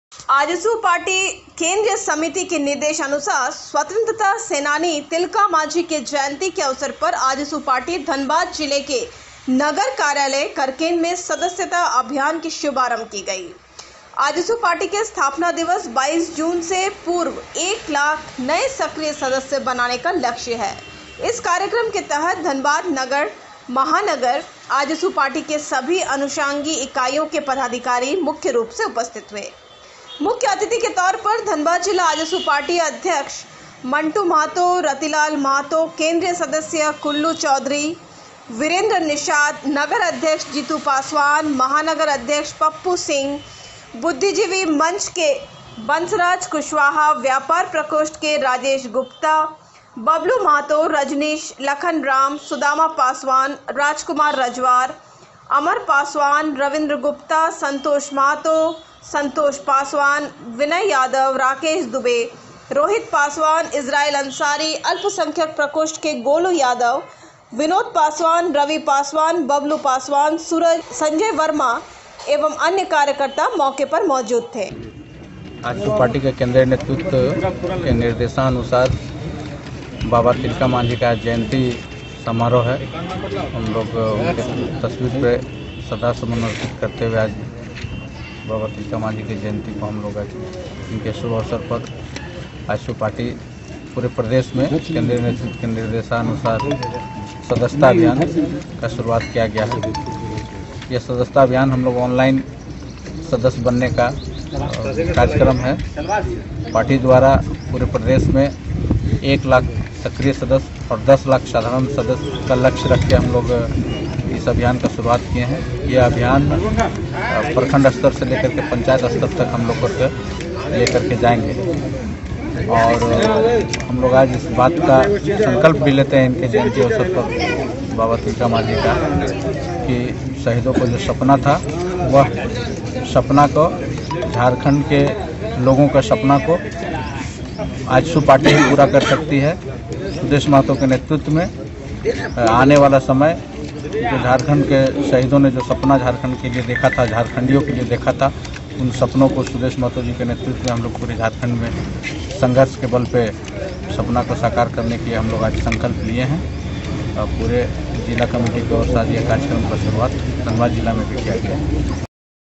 Aaj ka sach news,,11.02.2021
आजसू पार्टी केंद्रीय समिति के निर्देशानुसार स्वतंत्रता सेनानी तिलका मांझी के जयंती के अवसर पर आजसू पार्टी धनबाद जिला के नगर  कार्यालय करकेन्द में सदस्यता अभियान की शुभारंभ की गई। आजसु पार्टी के स्थापना दिवस 22 जून से पूर्व एक लाख नये सक्रिय सदस्य बनाने का लक्ष्य है इस कार्यक्रम के तहत धनबाद नगर, महानगर,  आजसू पार्टी के सभी अनुषंगी इकाइयों के पदाधिकारी मुख्य रूप से उपस्थित हुऐ,,, मुख्य अतिथि के तौर पर धनबाद जिला आजसू पाटी अध्यक्ष मंटू महतो ,रतीलाल महतों केंद्रीय सदस्य ,कुल्लू चौधरी वीरेंद्र निषाद नगर अध्यक्ष जीतु पासवान, महानगर अध्यक्ष पप्पू सिंह, बुद्धिजीवी मंच के बंसराज कुशवाहा व्यापार प्रकोष्ठ के राजेश गुप्ता बबलू महतों रजनीश लखन राम ,सुदामा पासवान, राजकुमार रजवार, अमर पासवान, रविंद्र गुप्ता ,संतोष महतो, संतोष पासवान, विनय यादव राकेश दुबे, रोहित पासवान, इजराइल अंसारी अल्पसंख्यक प्रकोष्ठ के गोलू यादव, विनोद पासवान, रवि पासवान, बबलू पासवान, सूरज संजय वर्मा एवं अन्य कार्यकर्ता मौके पर मौजूद थे